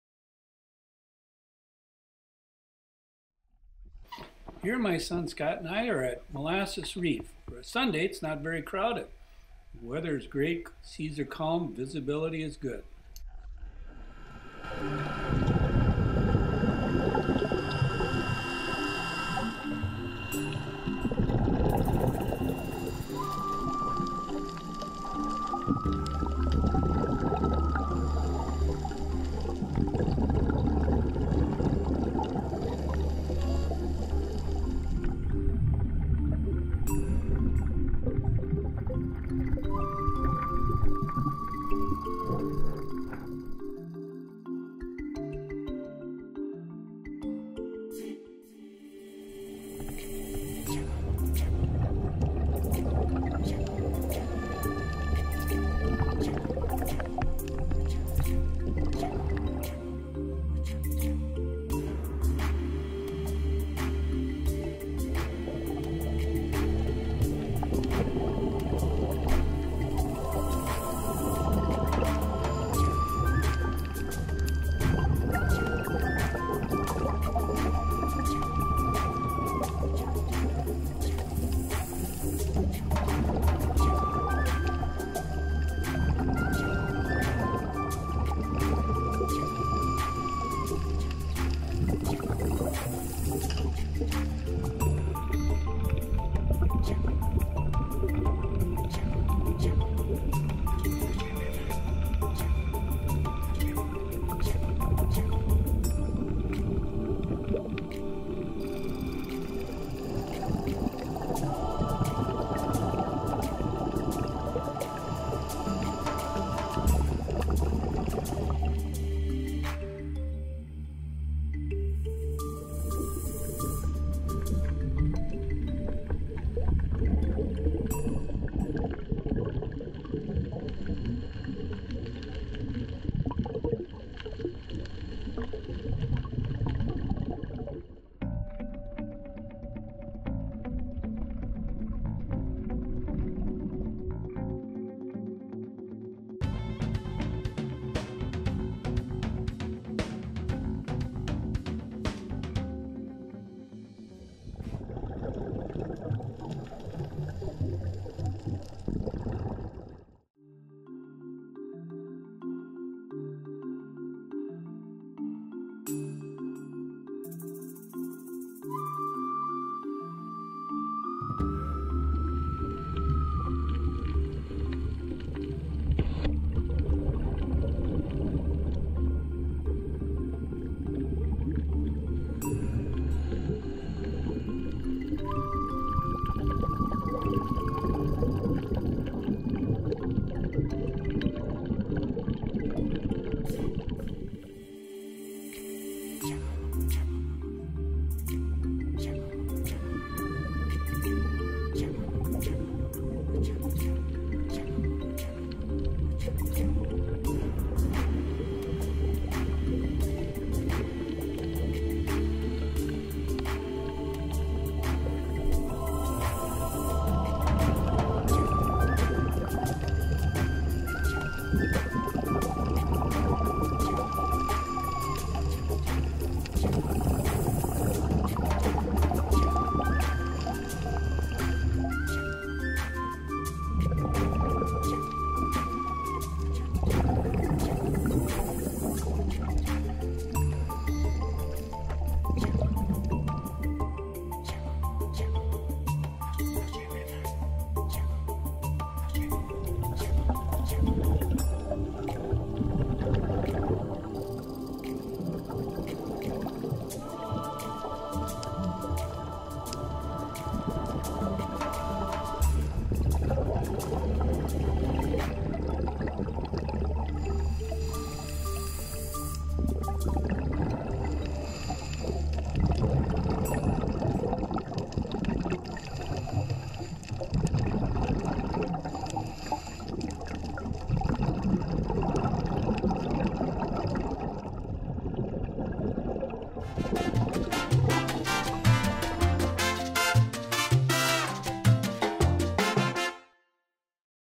Diving the NW section of Molasses Reef, Key Largo, FL
Lots of fish, an eel, a nurse shark and colorful coral and sponges at Molasses Reef in the Florida Keys Marine Sanctuary.